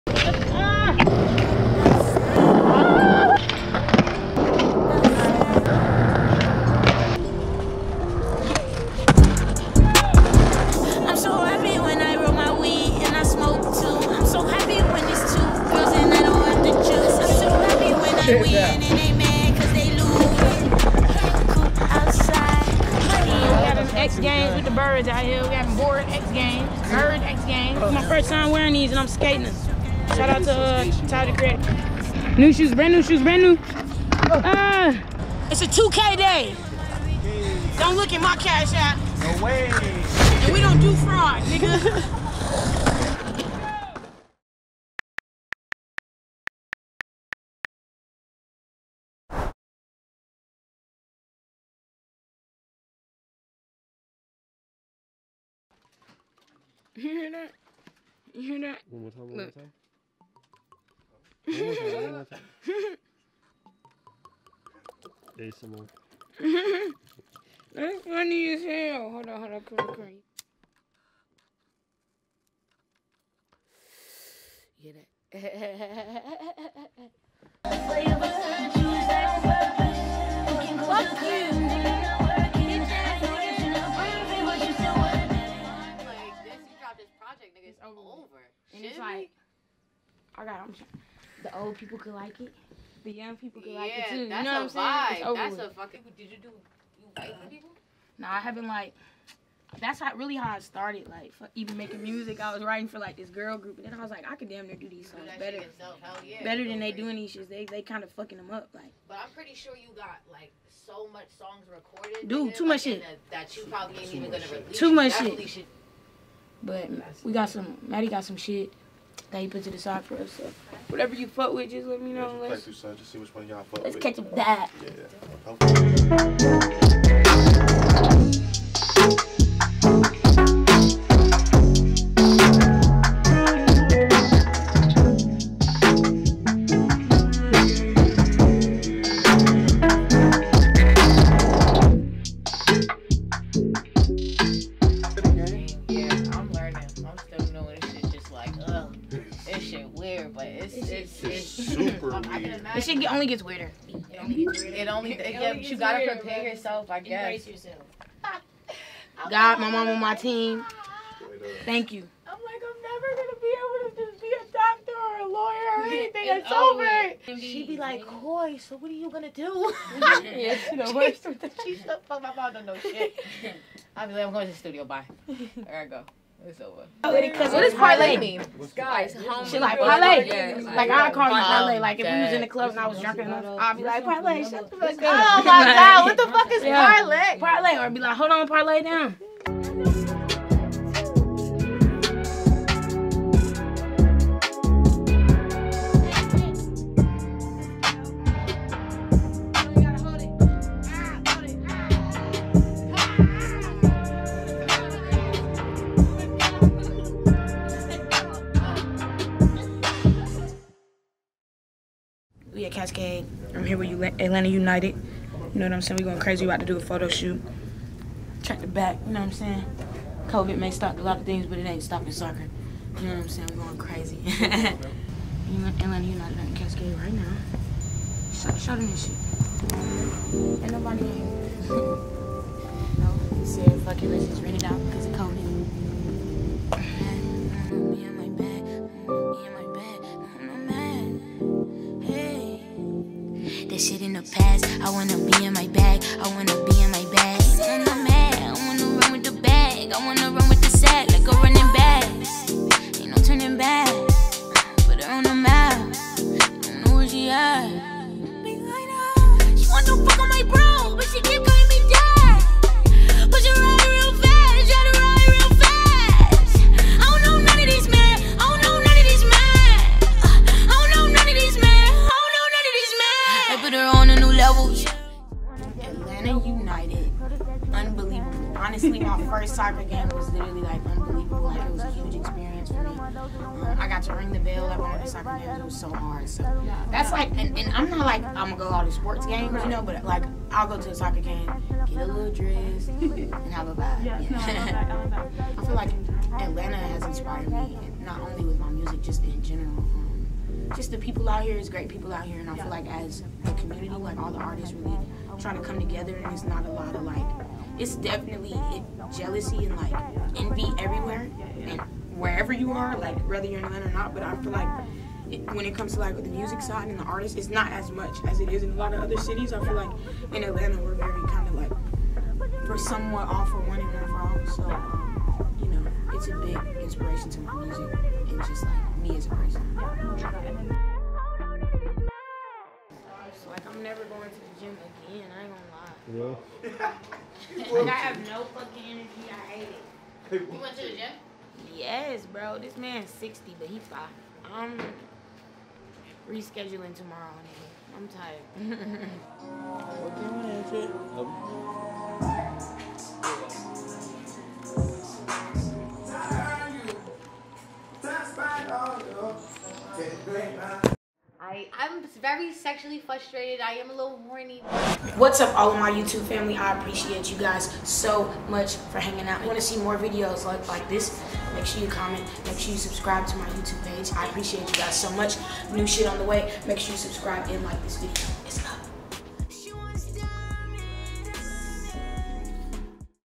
Slice of Life Ep. 1
Keep up to date with Kodie Shane as she shows you a slice of her life!